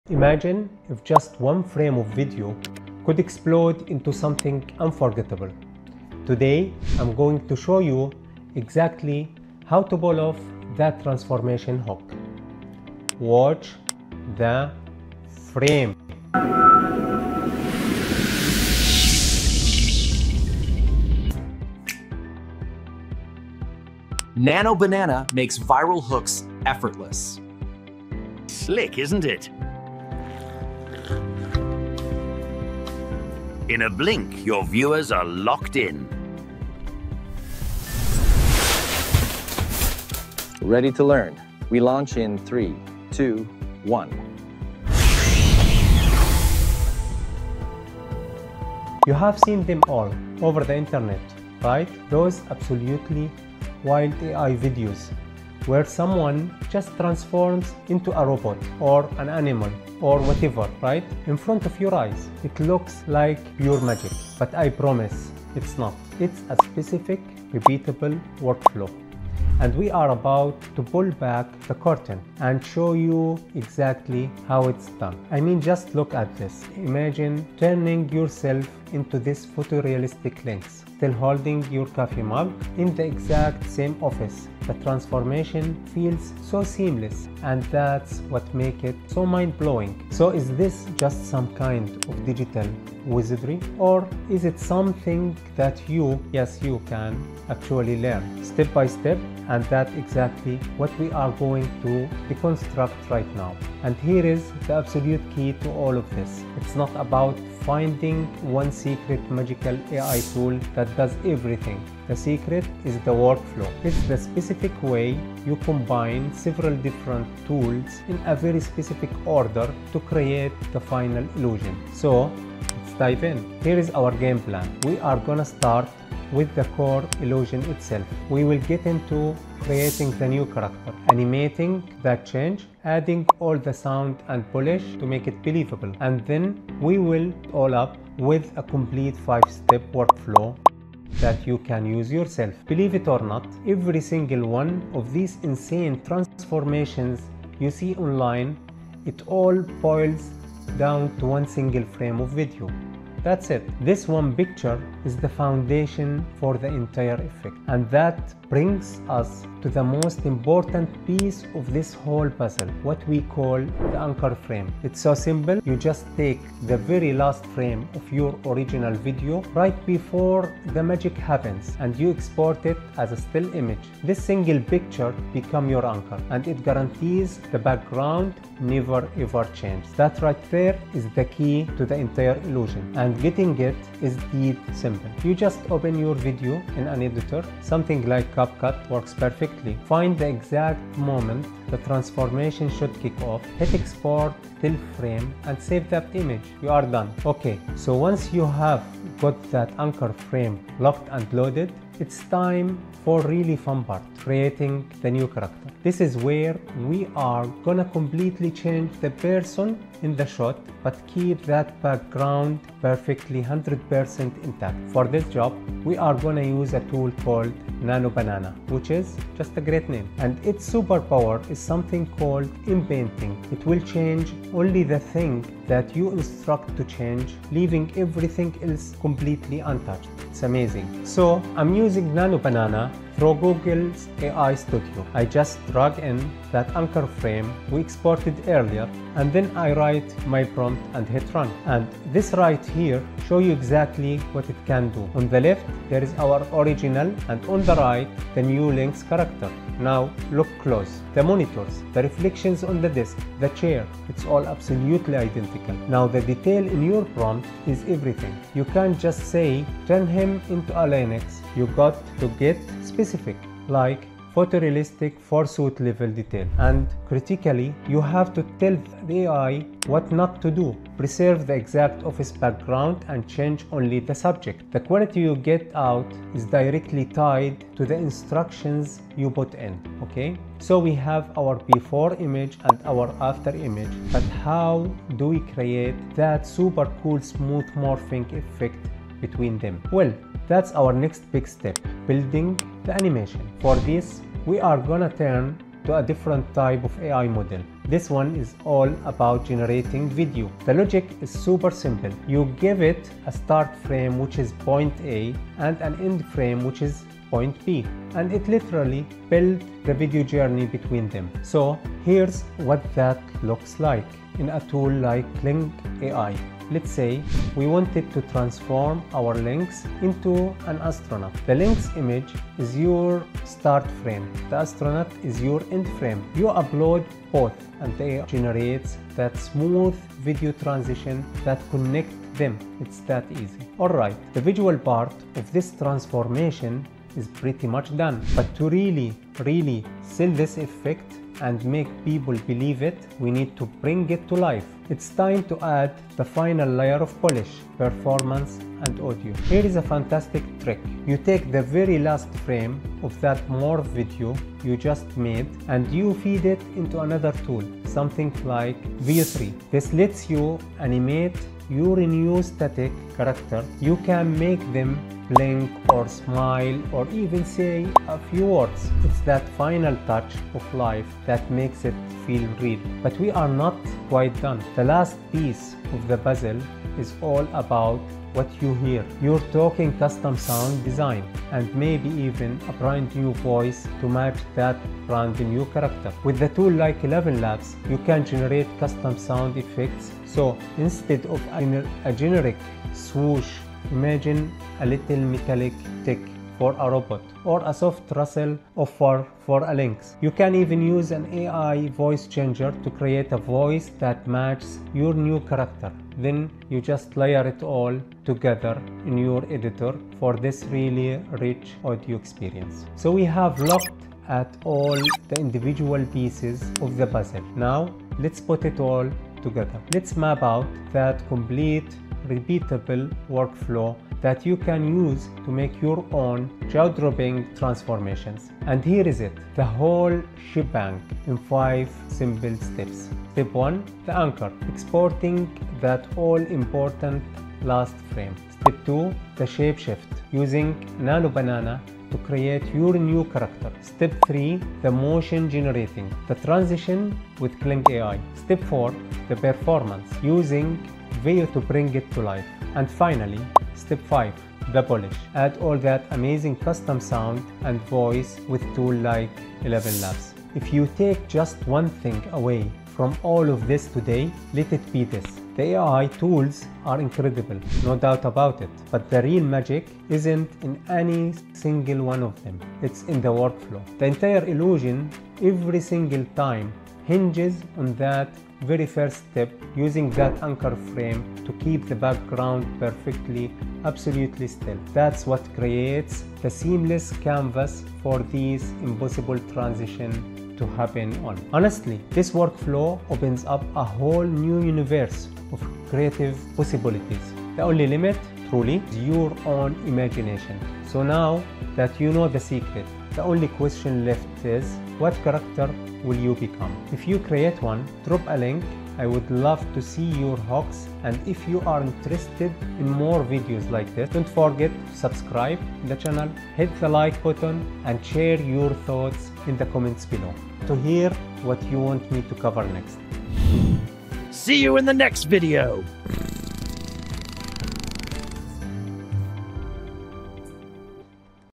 How One Frame Creates INSANE AI Transformations Full Workflow Revealed
What if one single frame of video could turn into a full AI transformation?
In this tutorial, I’m pulling back the curtain on the exact workflow behind those viral morphing videos you see all over the internet. You know the ones—where someone transforms into a robot, an animal, or an entirely new character in a perfectly seamless shot. It looks like magic… but it’s not. And today, I’ll show you exactly how to do it.

Inside this video, you’ll learn:

The anchor frame secret (the trick behind every seamless transformation)

How to use Nano Banana to reshape your character with perfect background preservation

How to build smooth transitions using Kling.AI

How to animate your new character with VEO3

How to add sound, voice, and realism using ElevenLabs

And the complete five-step workflow you can use to create ANY transformation you imagine

This is the real behind-the-scenes process that creators use to make viral AI videos — and once you understand the workflow, you can create anything your imagination dreams up.

If you create your own transformation, drop a link in the comments!